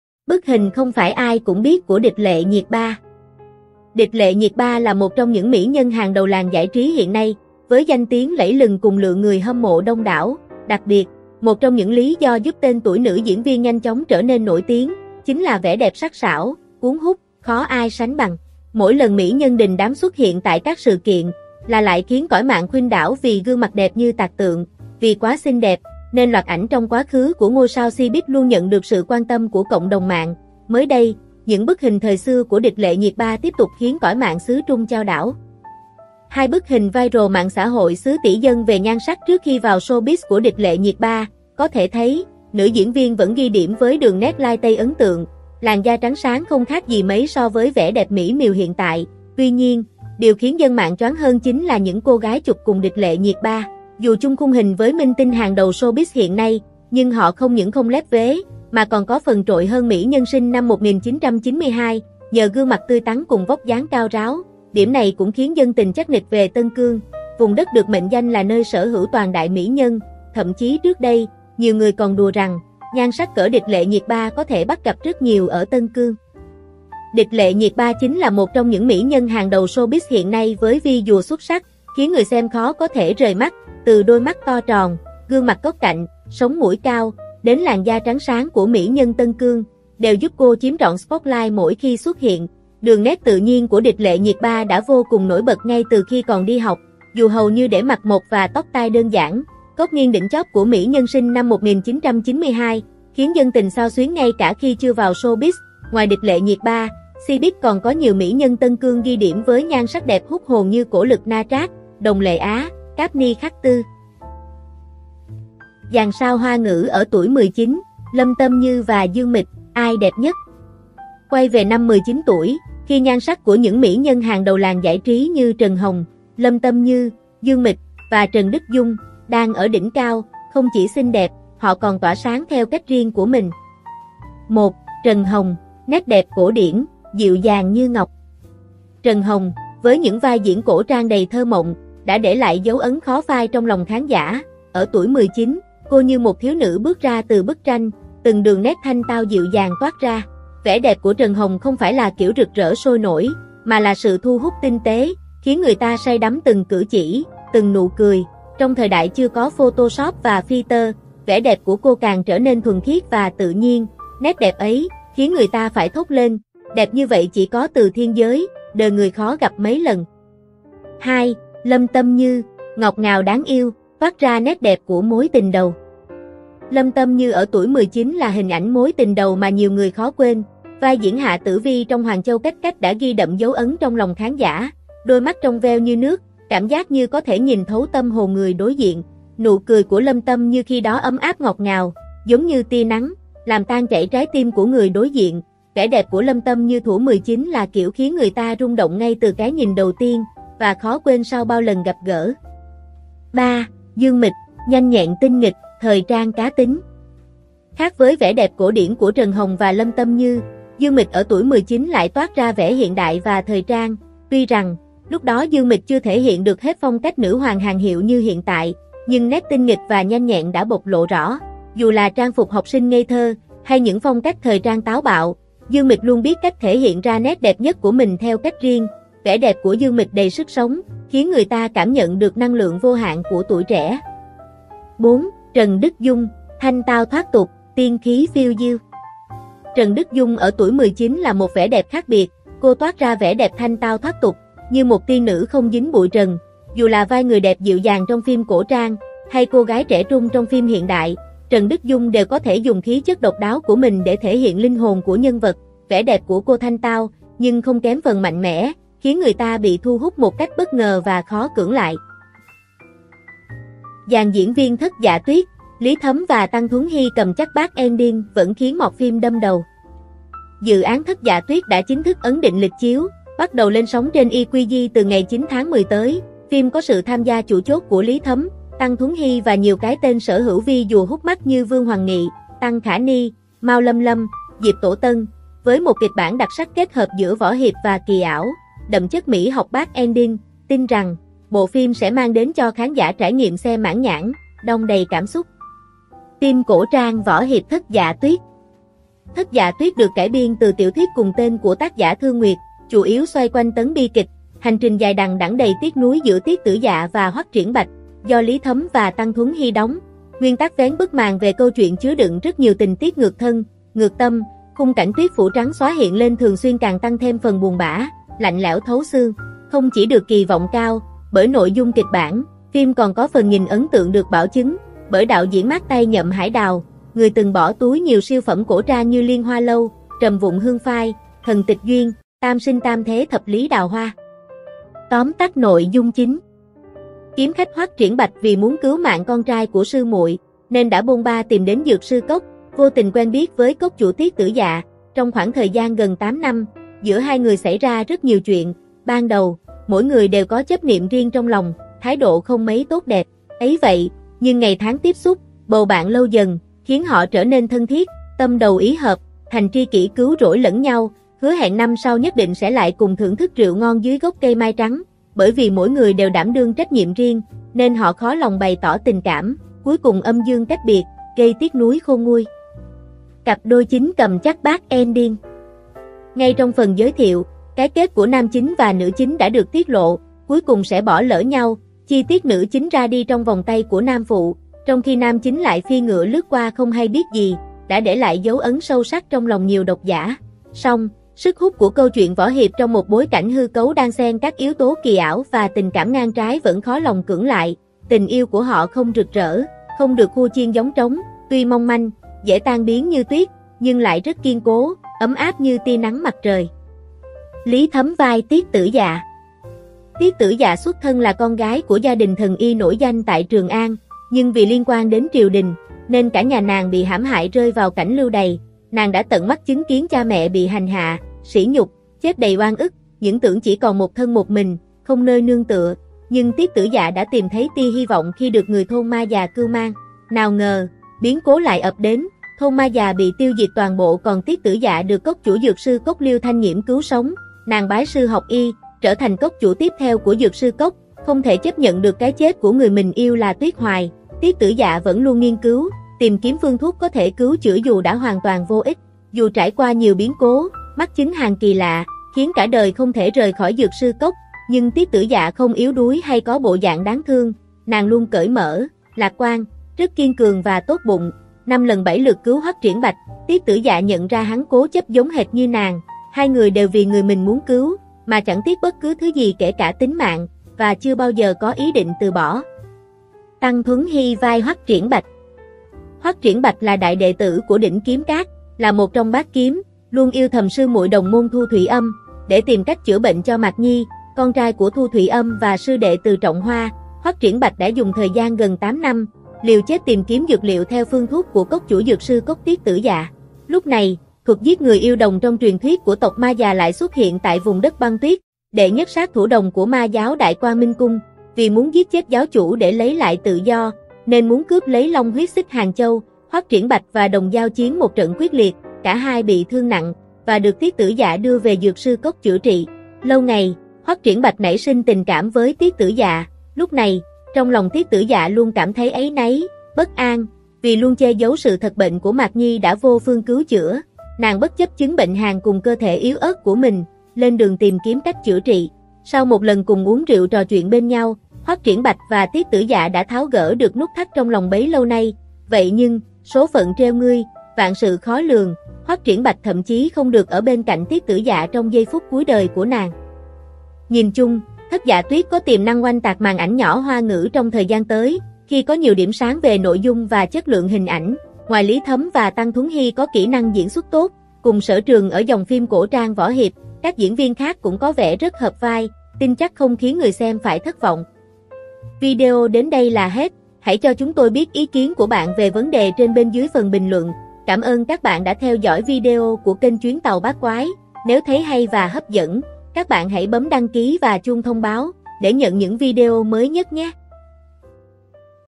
Lý Thấm cầm chắc BE vẫn khiến mọt phim đâm đầu, Dương Mịch Nhanh nhẹn tinh nghịch đầy cá tính
1. Bức hình không phải ai cũng biết của Địch Lệ Nhiệt Ba.
Địch Lệ Nhiệt Ba là một trong những mỹ nhân hàng đầu làng giải trí hiện nay với danh tiếng lẫy lừng cùng lượng người hâm mộ đông đảo. Đặc biệt, một trong những lý do giúp tên tuổi nữ diễn viên nhanh chóng trở nên nổi tiếng chính là vẻ đẹp sắc sảo, cuốn hút khó ai sánh bằng.
2. Dàn sao Hoa ngữ ở tuổi 19: Trần Hồng, Lâm Tâm Như, Dương Mịch và Trần Đức Dung, ai đẹp nhất?
Quay về năm 19 tuổi, khi nhan sắc của những mỹ nhân hàng đầu làng giải trí như Trần Hồng, Lâm Tâm Như, Dương Mịch, và Trần Đức Dung đang ở đỉnh cao. Không chỉ xinh đẹp, họ còn tỏa sáng theo cách riêng của mình.
3. Dàn diễn viên Thất Dạ Tuyết: Lý Thấm - Tăng Thuấn Hy cầm chắc BE vẫn khiến mọt phim đâm đầu.
Dự án Thất Dạ Tuyết đã chính thức ấn định lịch chiếu, bắt đầu lên sóng trên iQIYI từ ngày 9/10 tới. Phim có sự tham gia chủ chốt của Lý Thấm, Tăng Thuấn Hy và nhiều cái tên sở hữu visual hút mắt như Vương Hoằng Nghị, Tăng Khả Ny, Mao Lâm Lâm, Diệp Tổ Tân... Với một kịch bản đặc sắc kết hợp giữa võ hiệp và kỳ ảo đậm chất mỹ học BE, tin rằng bộ phim sẽ mang đến cho khán giả trải nghiệm xem mãn nhãn, đong đầy cảm xúc.
Cảm ơn các bạn đã theo dõi video của kênh Chuyến tàu bát quái.